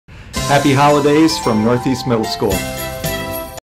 Happy Holidays from Northeast Middle School
Happy Holidays from Assistant Principal Keith T Seybert!

Watch all our Holiday Videos here in our Happy Holidays Playlist or tune into our tv channel, Charter channel 190 and AT&T Channel 99,  between programming for our Holiday video Mashups!

This has been a production of MPSTV through Midland Public Schools